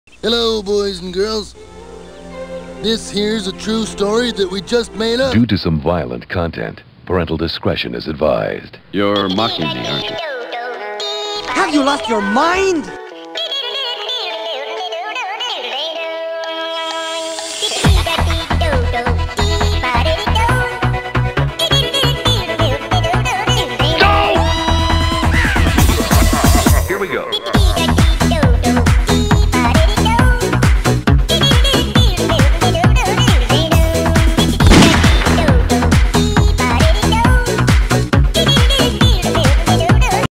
Last Lego battle
Two legos have a silly battle. Soon a robot attacks the two warriors and kills them.